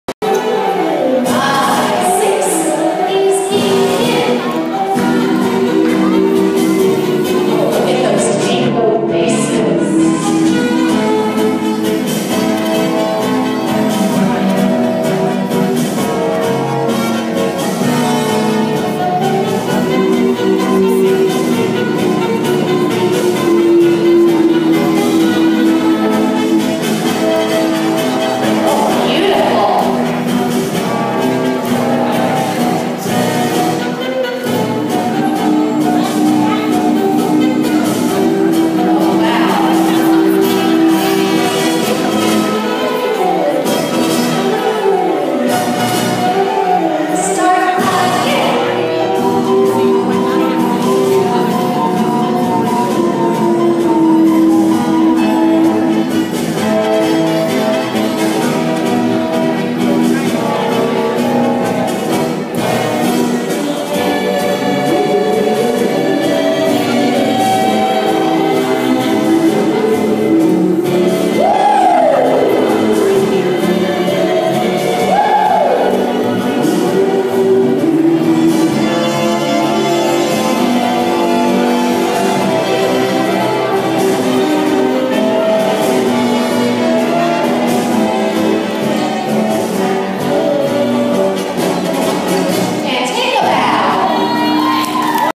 Bettirose in Ballroom Dance Competition Tango
At PS114 on 11 Dec 2014